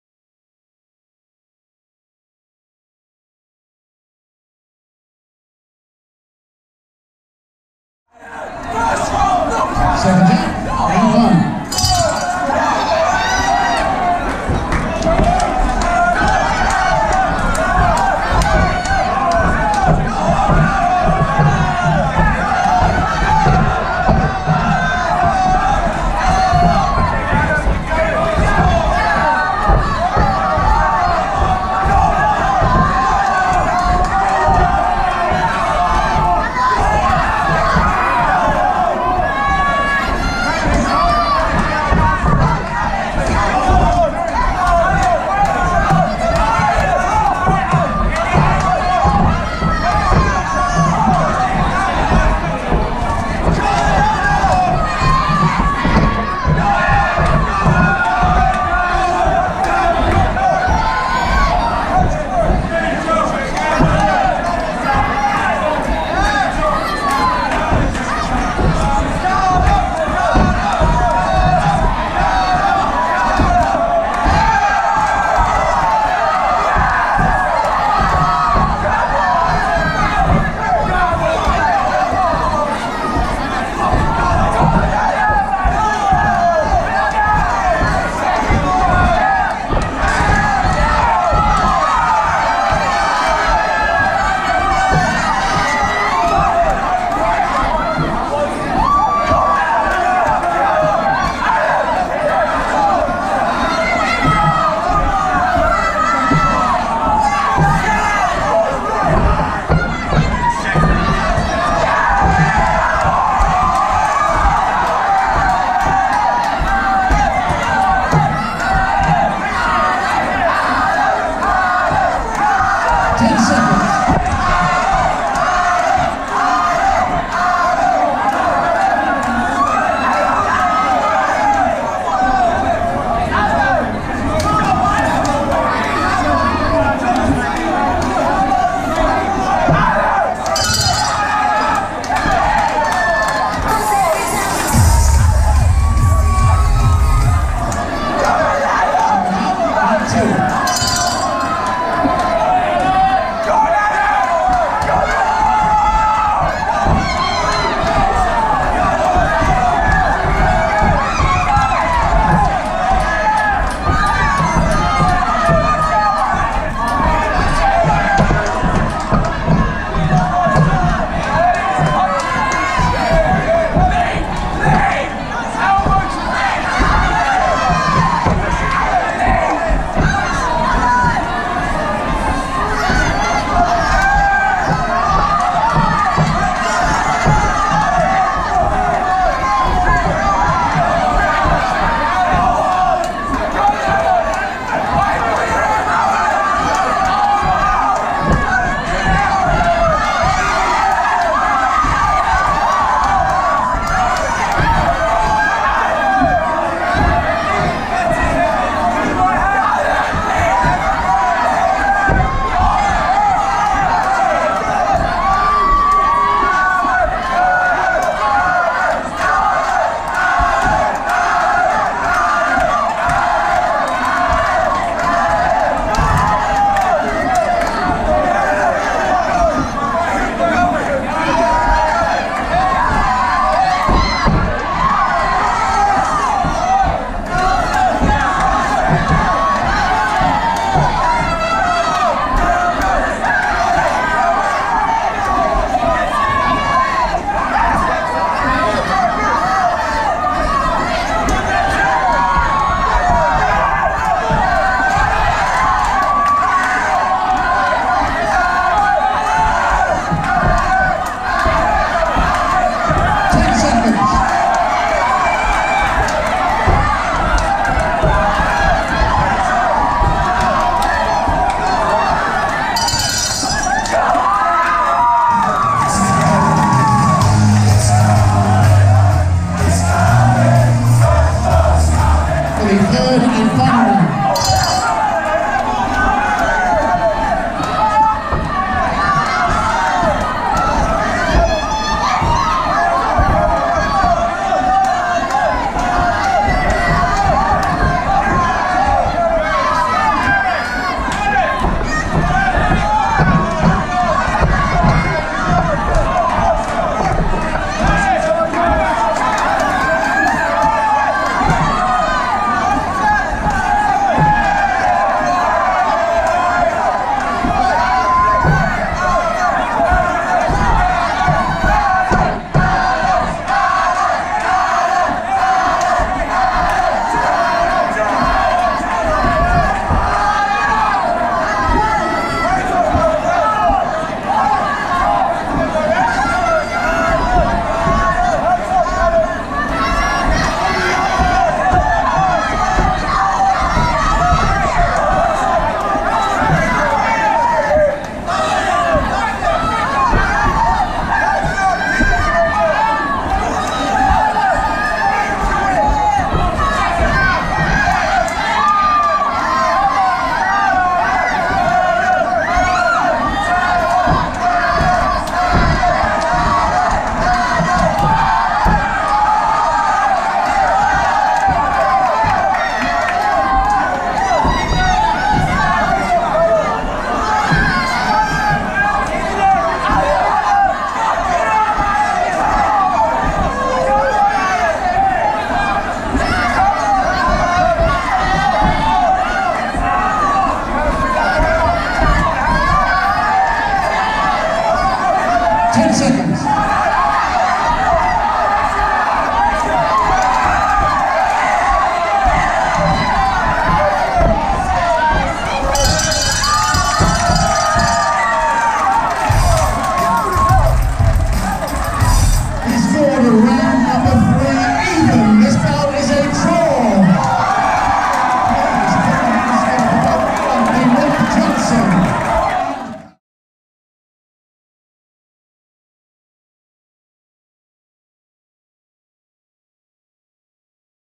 Ultra White Collar Boxing | Luton | Alan Johnson VS Callum Chase
Luton Ultra White Collar Boxing 7th July 2018

8 Weeks FREE training / Get in the best shape of your life/ Raise money for Cancer Research UK